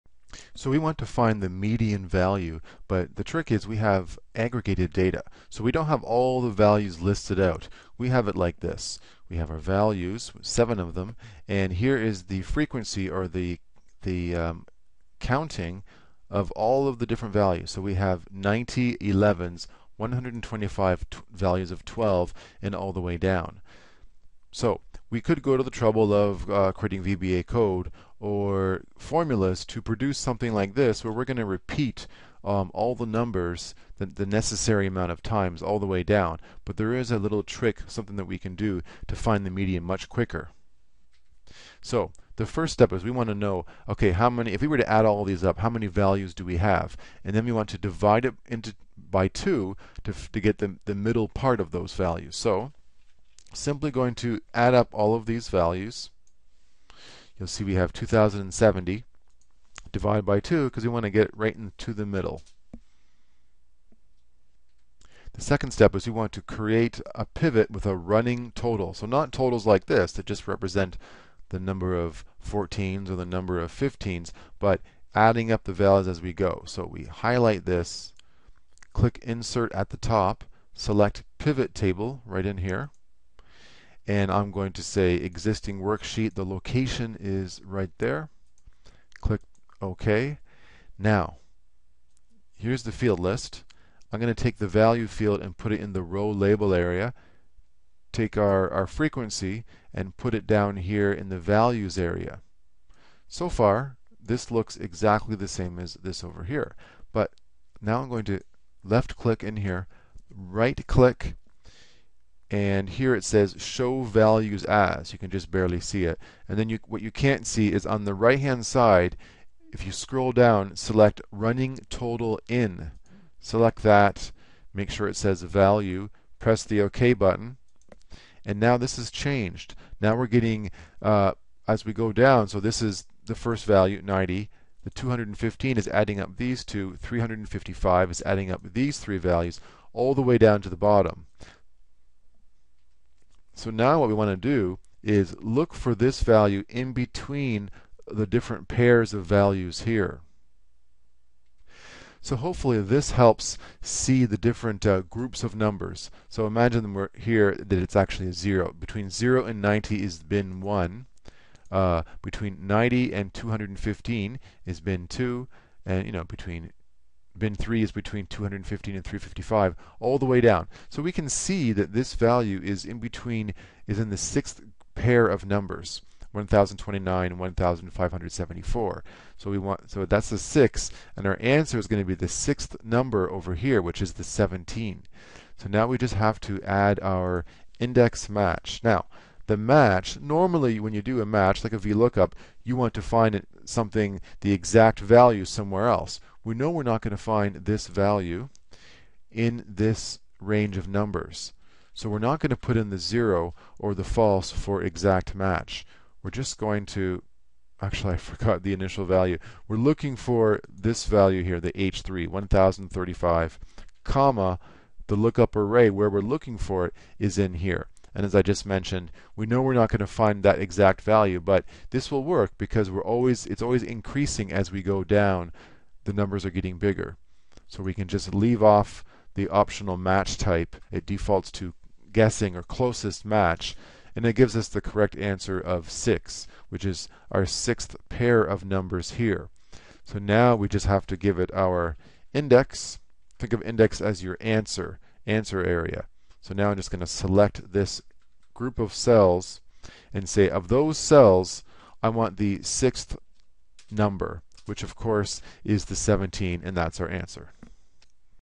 Video 00077 Find the MEDIAN value in Excel with only aggregated data
If you only have the aggregated data in Excel you might be tempted to create all of the data to calculate the median.  There is a much easier way!
In this video I show you the 3 steps.

Cheers,
Kevin Lehrbass